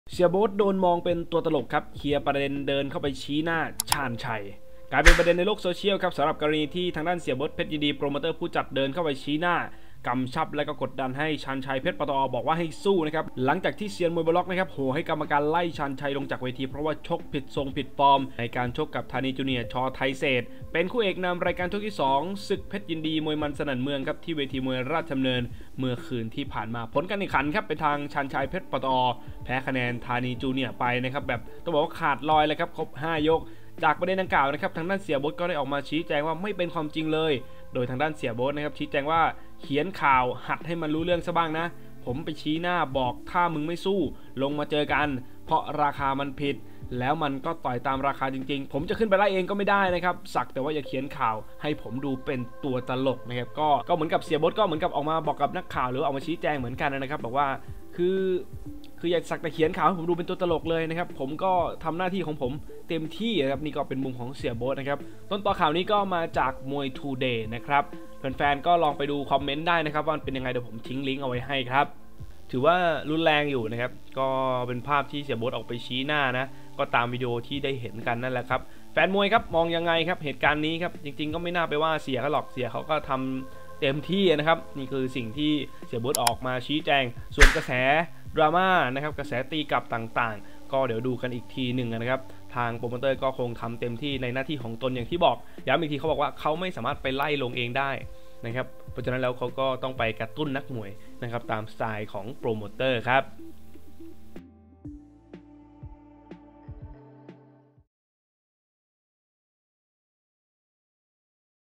เคลียร์ประเด็นเดินเข้าไปชี้หน้า ชาญชัย หลังจากที่เซียนมวยบนล็อคโห่ให้กรรมการไล่
ข่าวจาก มวยทูเด

เสี่ยโบ๊ท โดนมองเป็นตัวตลก เคลียร์ประเด็นเดินเข้าไปชี้หน้า
กลายเป็นประเด็นในโลกโซเชียลสำหรับกรณีทางด้าน เสี่ยโบ๊ท เพชรยินดี โปรโมเตอร์ผู้จัด เดินเข้าไปชี้หน้า กำชับเเละกดดัน ชาญชัย เพชร ปตอ. บอกให้สู้  หลังจากที่เซียนมวยบนล็อคโห่ให้กรรมการไล่ ชาญชัยลงจากเวที เพราะชกไม่ผิดทรง ผิดฟอร์ม ในการชกกับ ธานีจูเนียร์  ช.ไทยเศรษฐ์ เป็นคู่เอกนำรายการช่วงที่2 ของศึกเพชรยินดีมวยมันส์สนั่นเมือง ที่เวทีมวยราชดำเนิน เมื่อช่วงค่ำที่ผ่านมา ผลการเเข่งขัน ชาญชัย  เพชร ปตอ. แพ้คะแนน ธานีจูเนียร์  ช.ไทยเศรษฐ์ ไปเเบบขาดลอยเมื่อชกกันครบ5ยก
จากประเด็นดังกล่าวนั้นทางด้านเสี่ยโบ๊ทได้ออกมาชี้แจงว่าไม่เป็นความจริงเลย โดยทางด้านเสี่ยโบ๊ทได้ ชี้แจงว่า เขียนข่าวหัดให้มันรู้เรื่องบ้างนะ ผมไปชี้หน้าบอกถ้ามึงไม่สู้ ลงมาเจอกัน เพราะราคามันผิด แล้วมันก็ต่อยตามราคาจริงๆ แต่กรรมการไม่ยอมไล่ ผมจะขึ้นไปไล่เองก็ไม่ได้ อย่าสักแต่เขียนข่าว ให้ผมดูเป็นตัวตลก